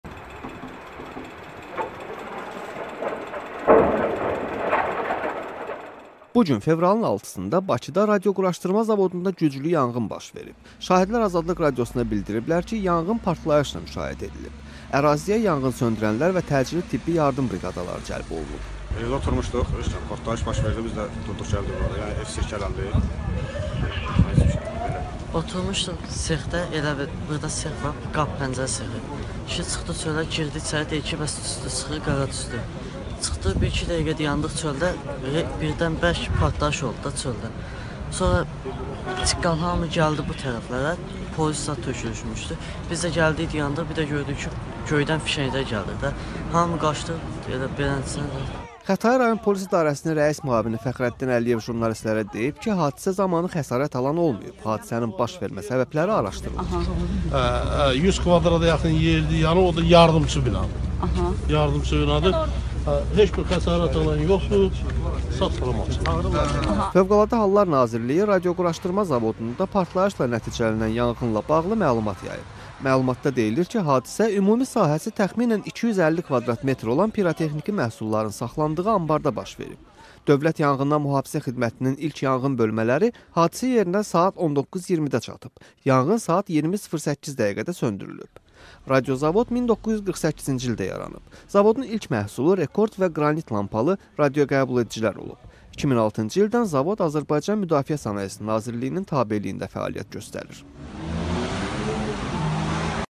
Bakıda partlayış olub - REPORTAJ
Müdafiə Sənayesi Nazirliyinin tabeçiliyində olan "Radiozavod"dakı partlayış yanğınla müşayiət olunub.